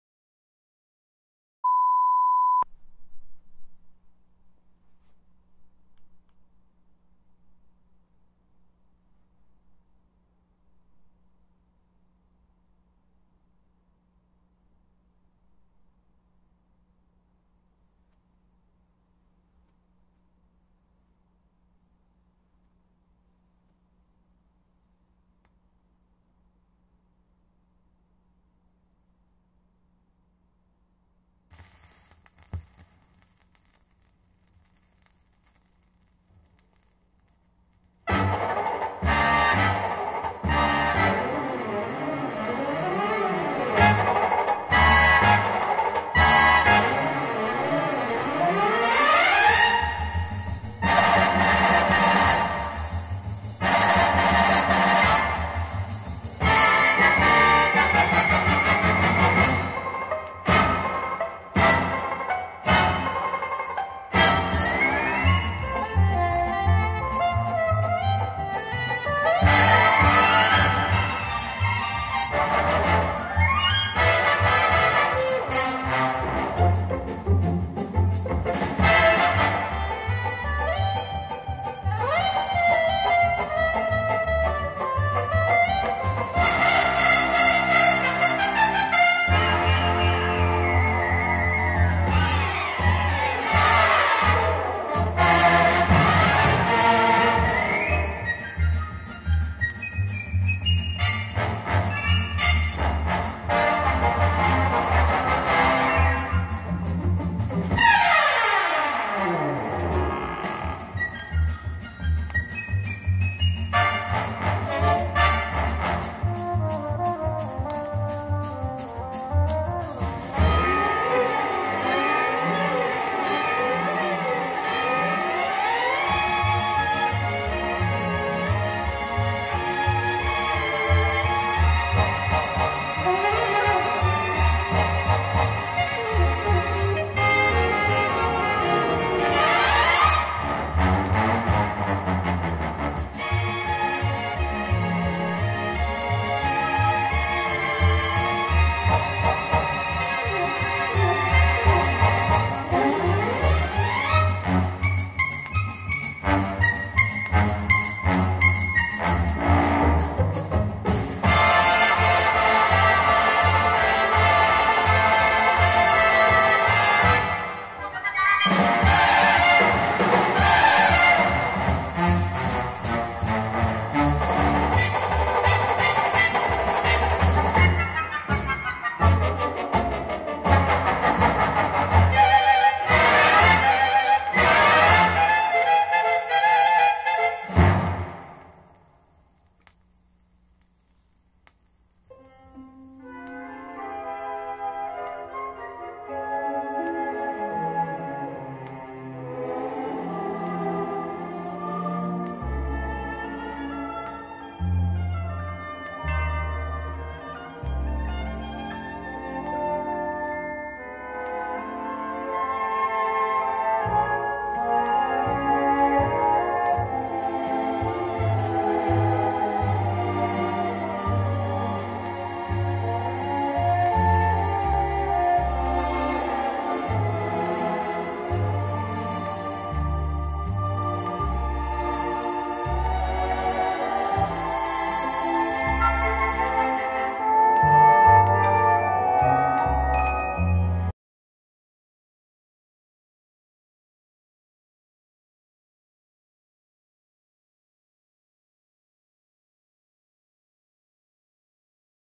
Model ZF15M speaker demo
Zen Master Series open baffle from Decware playing full orchestra music.  Driven by 2 watt SET amplification.